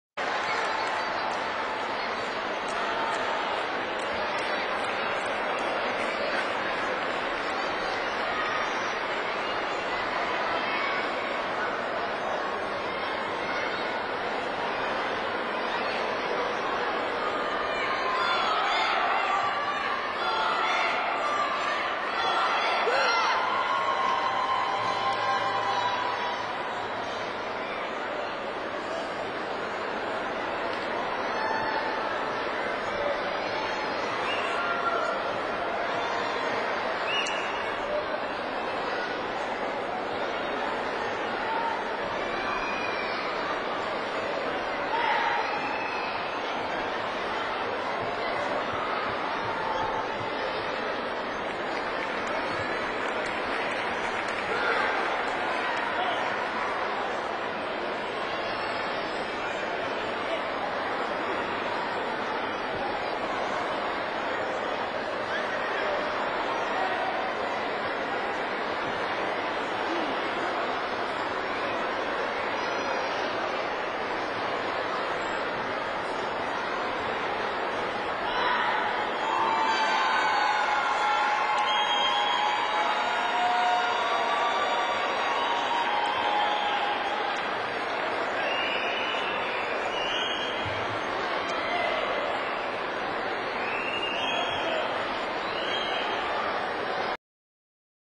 3rd WTF World Taekwondo Poomsae Championships 2008 1st Pair Q.Final Uzbekistan R 2
3rd WTF World Taekwondo Poomsae Championships 2008 1st Pair Q.Final Pan Irina, Li Alexandr (Uzbekistan) R 2